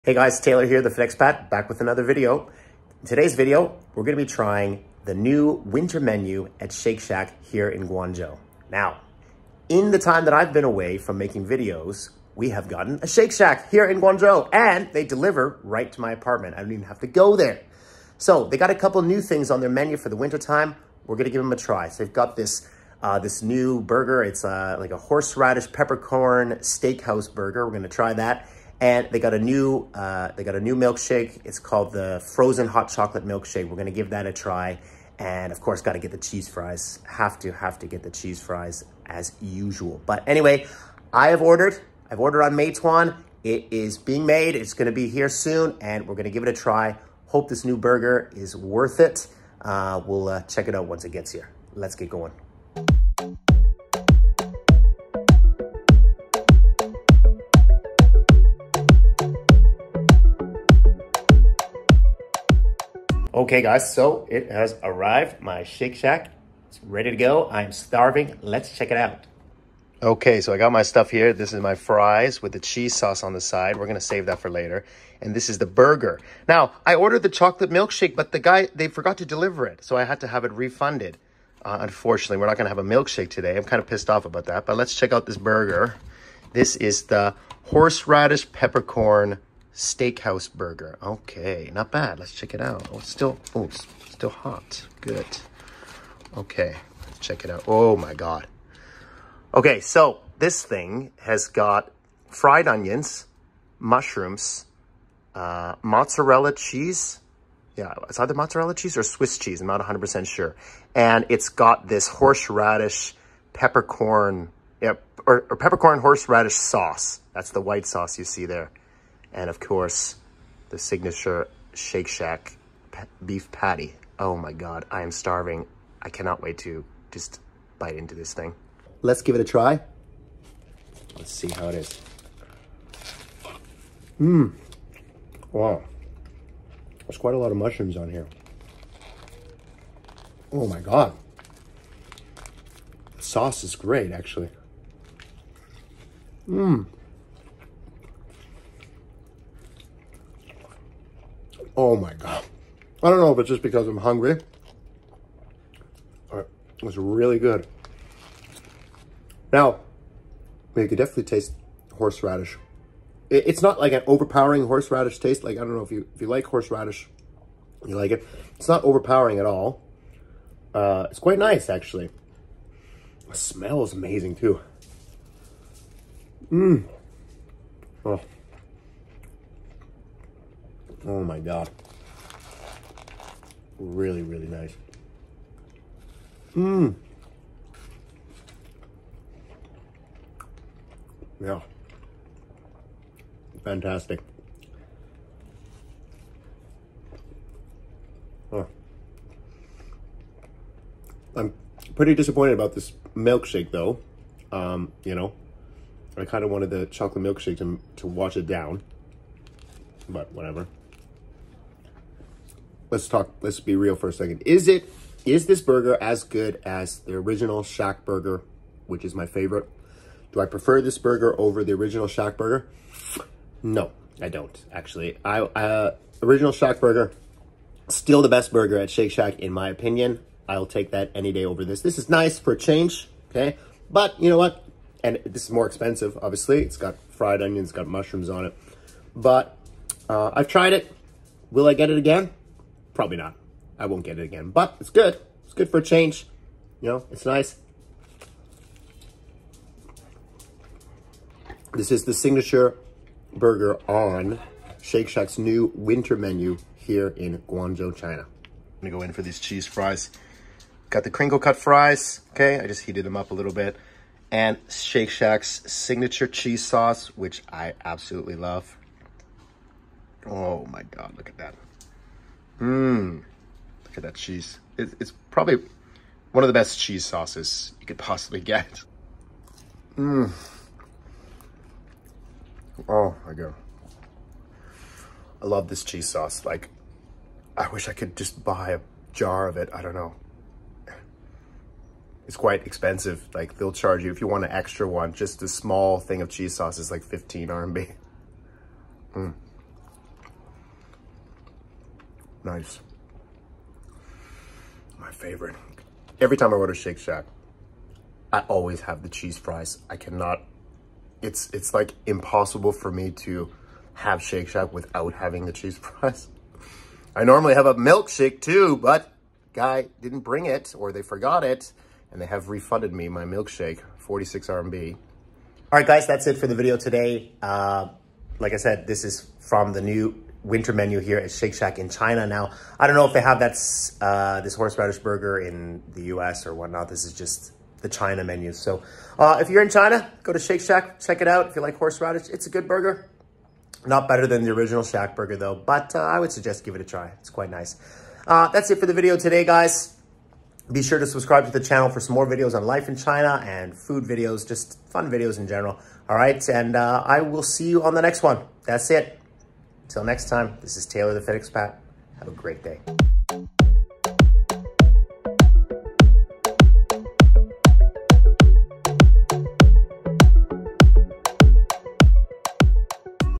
Shake Shack's New Winter Menu 2023
Shake Shack has absolutely exploded here in China in recent years. They are popping up in major cities all across the country. In today's video I will be checking out their new winter seasonal menu and trying their featured burger of the season. Check it out to see how it stacks up against their normal signature menu.